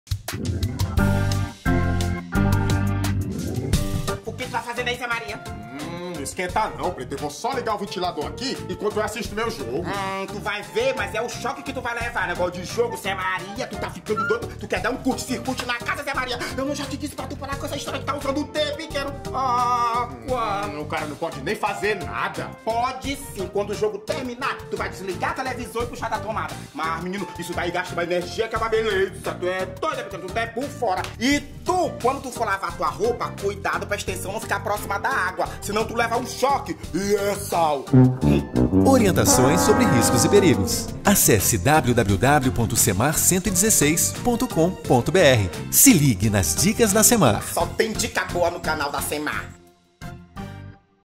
Segurança Cemar - Evite usar "T" ou Benjamin
O que está em jogo é a segurança da sua família, por isso não pise na bola. Evite utilizar o “T” ou Benjamin, pois isso pode sobrecarregar a tomada e ocasionar curto-circuitos.